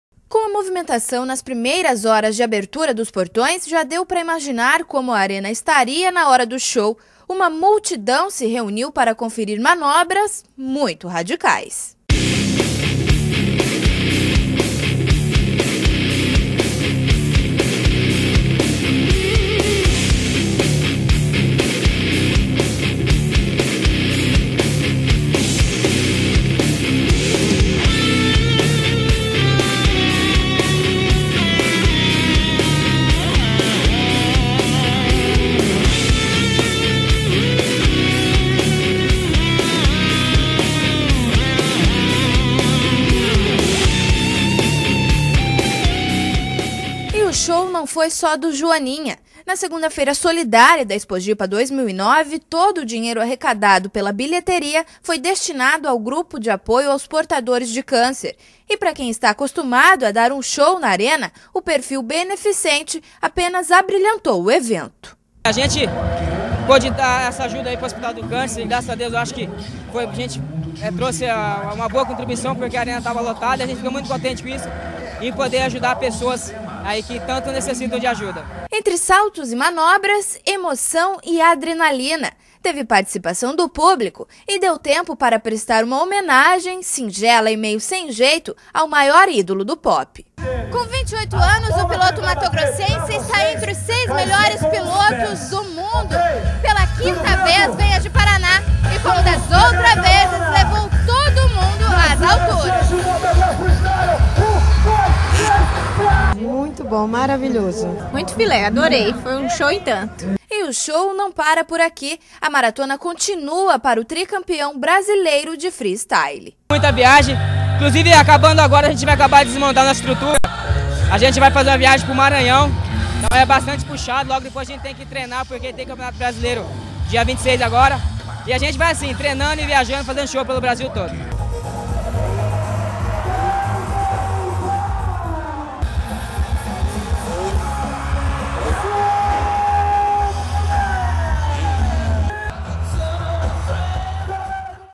JUNTO COM O PILOTO JOANINHA, POPULAÇÃO DÁ UM SHOW DE SOLIDARIEDADE NA EXPOJIPA.
SEGUNDA-FEIRA SOLIDÁRIA DA EXPOJIPA DÁ SHOW DE SOLIDARIEDADE E MANOBRAS RADICAIS. O FAMOSO PILOTO DE FREE STYLE, JOANINHA, VOLTOU A JI-PARANÁ, E TODO O DINHEIRO ARRECADADO NA BILHETERIA FOI DESTINADO AO GAPC, GRUPO DE APOIO AOS PORTADORES DE CÂNCER. REPORTAGEM KARINA QUADROS.